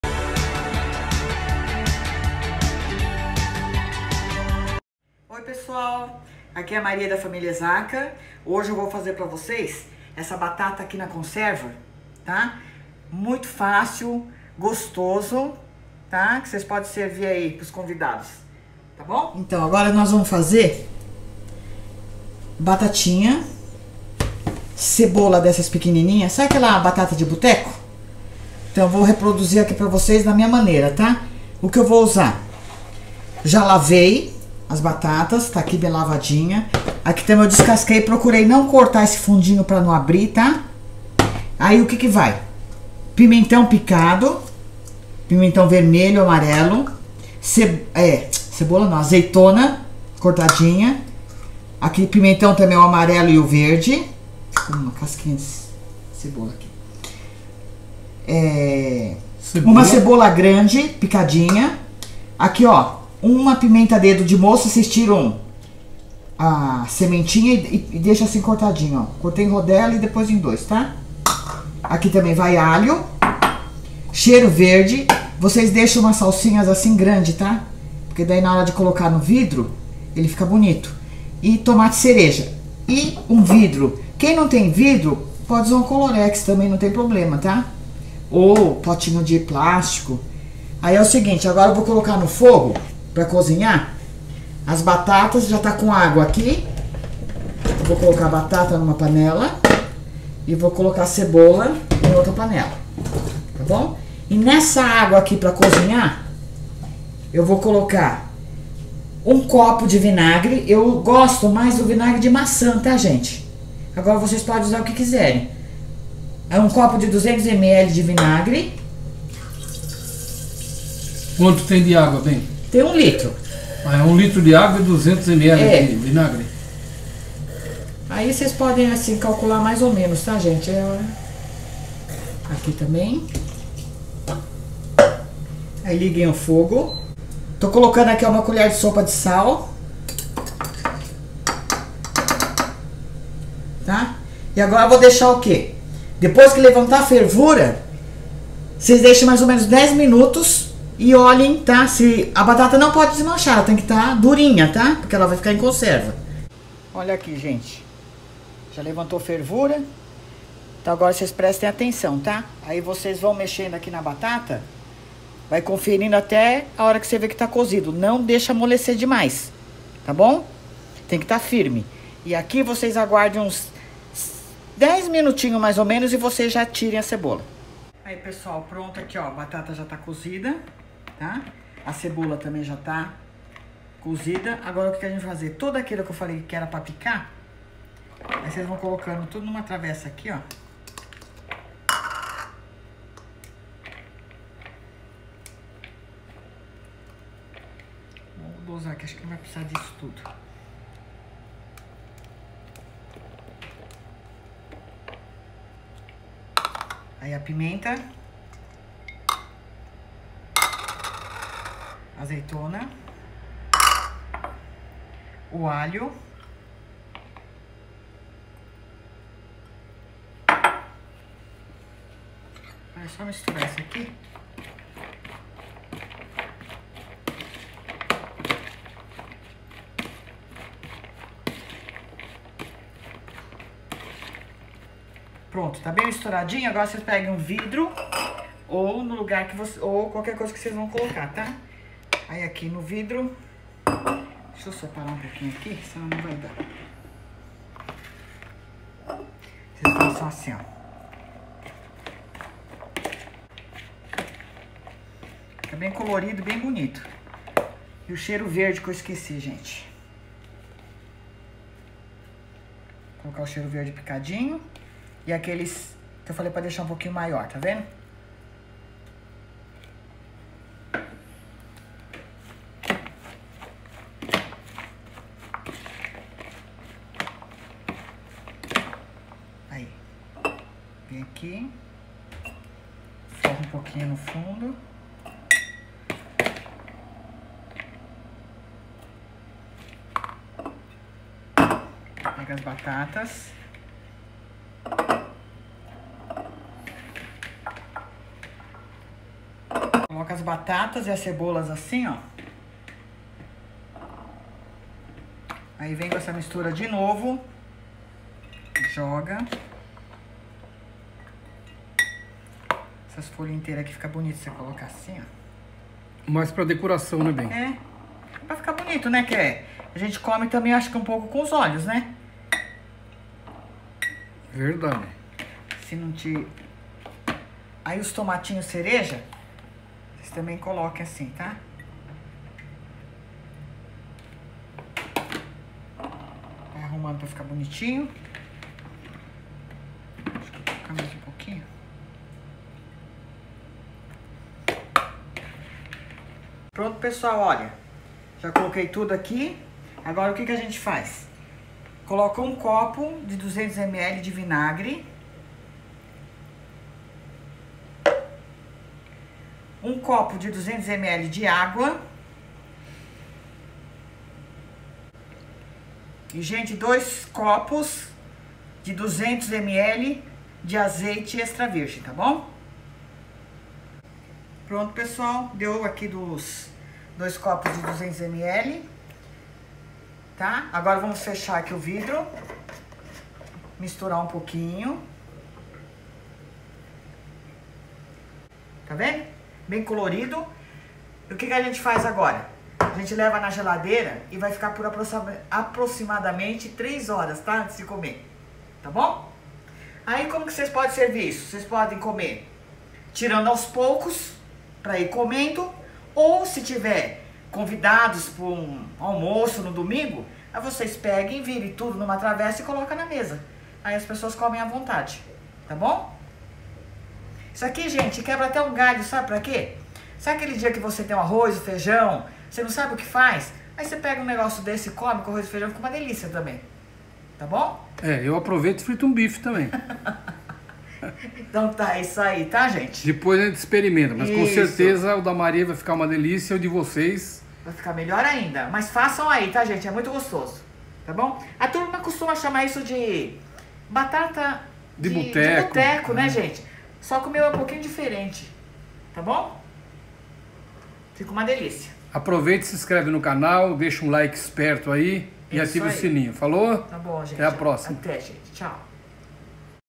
Batata de boteco, muito diferente
Batata
Cebola
Pimentão picado
Cebola
Pimenta dedo de moça
Alho
Cheiro verde
1 litro de agua
200 ml de vinagre
sal
400 ml azeite